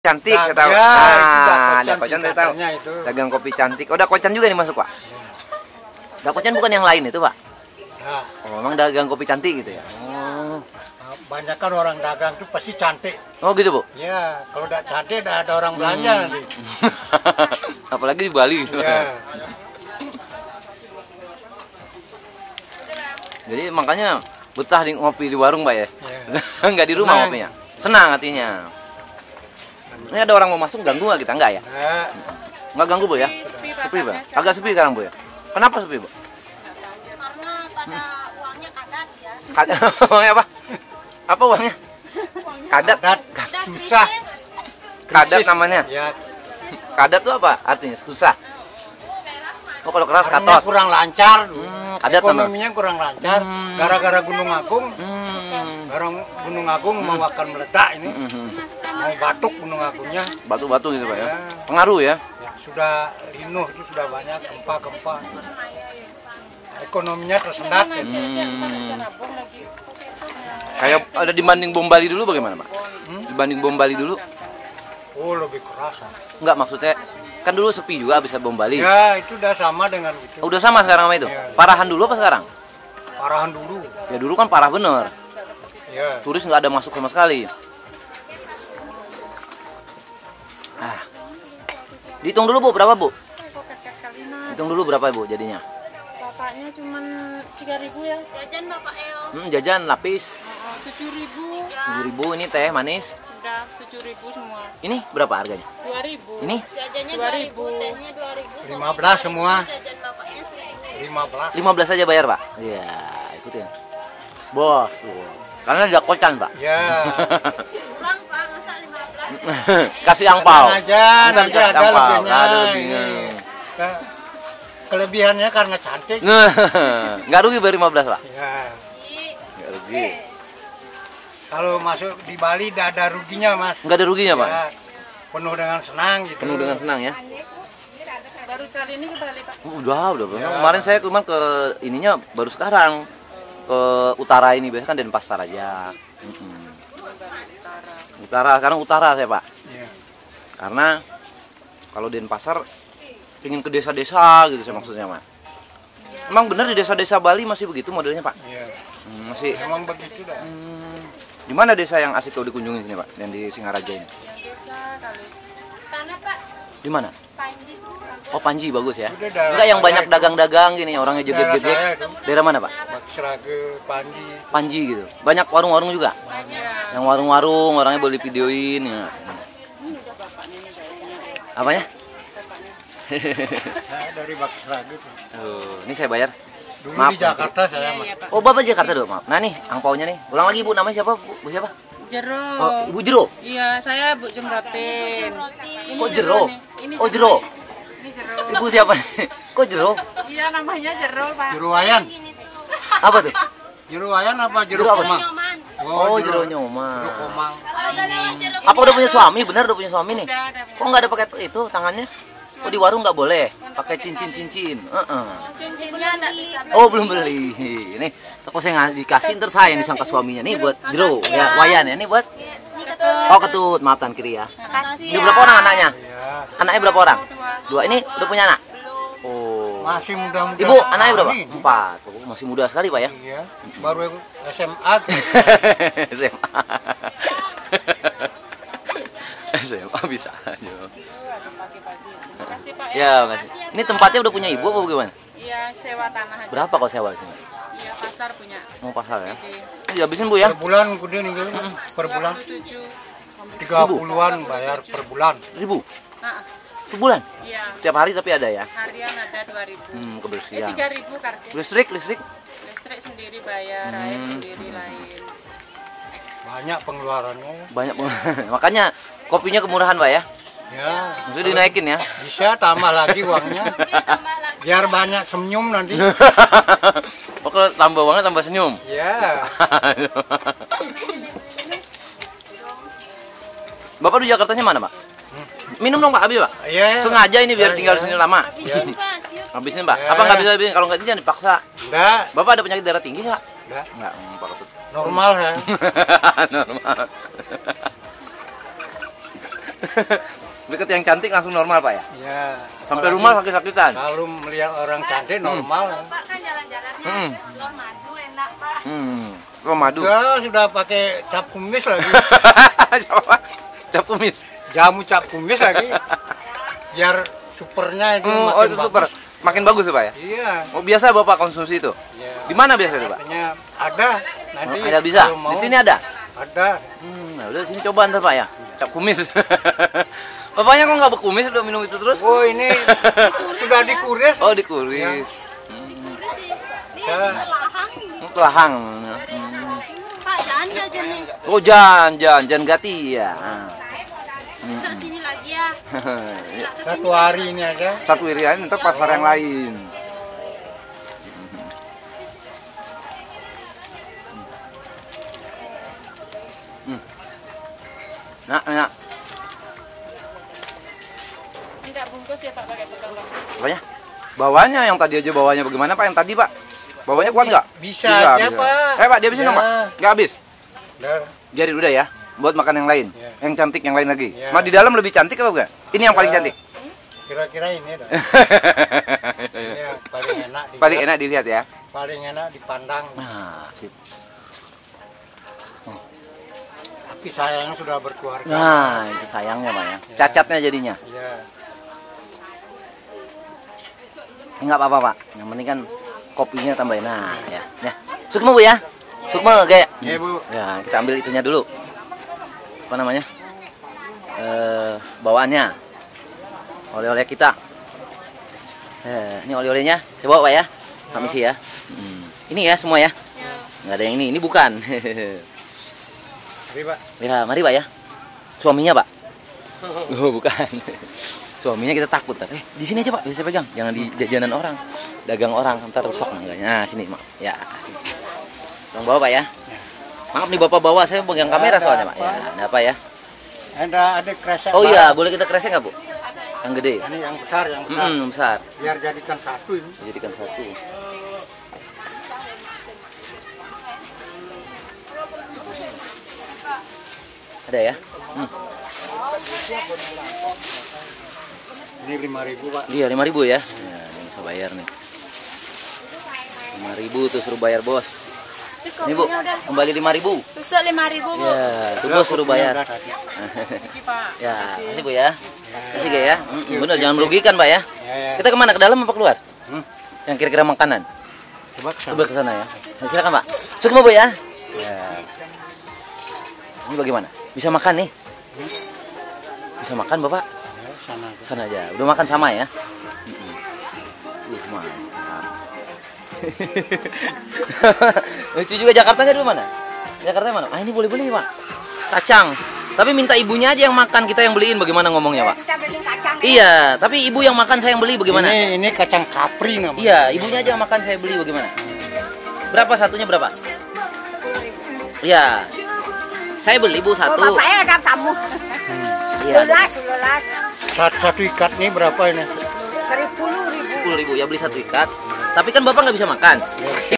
Indonesia Bali Street Food 2089 Part.2 Dakocan Dagang Kopi Cantik Pasar Anyar Singaraja Buleleng
We can buy   Dakocan Dagang Kopi Cantik only costs Rp.3.000 (23 Cents USD) per glass the taste is so good.